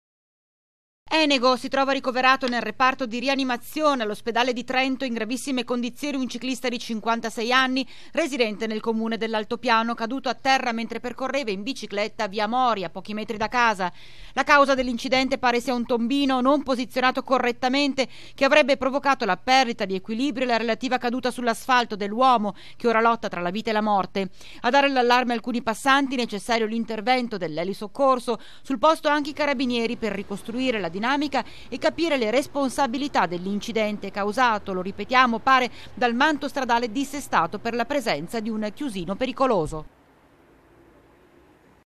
TG BASSANO (15/07/2019) - CADE A TERRA PER UN TOMBINO, GRAVISSIMO CICLISTA
TG BASSANO (lunedì 15 luglio 2019) - Enego. Si trova ricoverato nel reparto di rianimazione all'ospedale di Trento, in gravissime condizioni, un ciclista di 56 anni, residente nel comune dell'altopiano, caduto a terra mentre percorreva in bicicletta via Mori, a pochi metri da casa. La causa dell'incidente pare sia un tombino non posizionato correttamente, che avrebbe provocato la perdita di equilibrio e la relativa caduta sull'asfalto dell'uomo che ora lotta tra la vita e la morte. A dare l'allarme alcuni passanti. Necessario l'intervento dell'elisoccorso.  Sul posto anche i carabinieri, per ricostruire la dinamica e capire le responsabilità dell'incidente, causato, pare, lo ripetiamo, dal manto stradale dissestato per la presenza di un chiusino pericoloso.